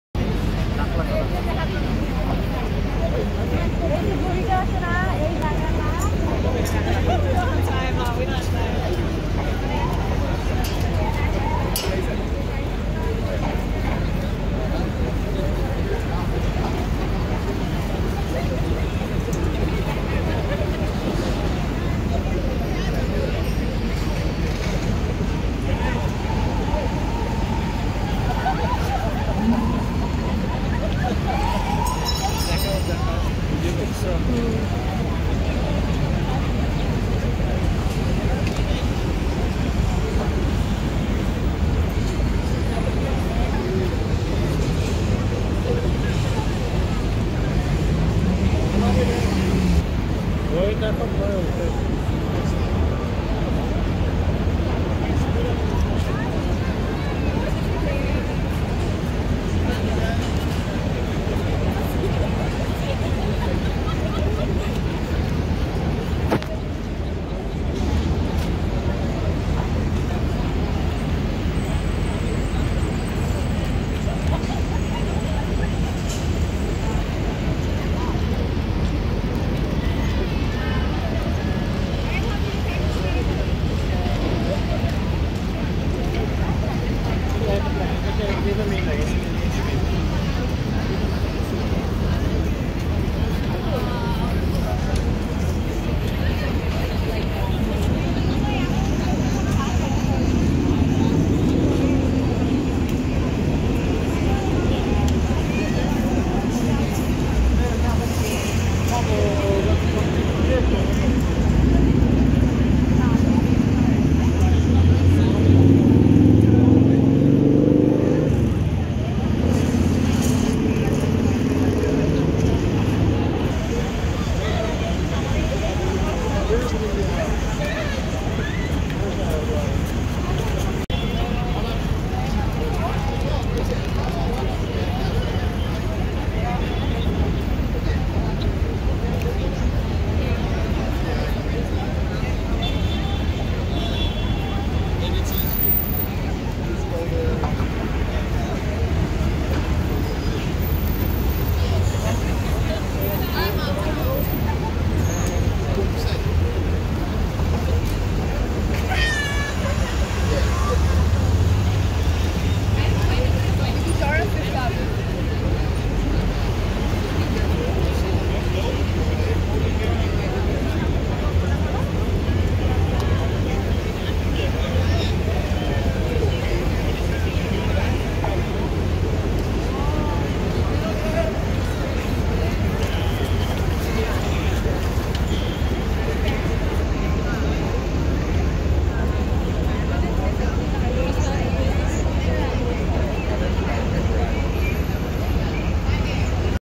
Time Square New York city#বিশ্বের সবচেয়ে বড় পর্দায় বিজ্ঞাপন প্রচারের শহর নিউইয়র্কের টাইমস্কয়ার#
Times Square is a major commercial intersection, tourist destination, entertainment center, and neighborhood in the Midtown Manhattan section of New York City, at the junction of Broadway and Seventh Avenue.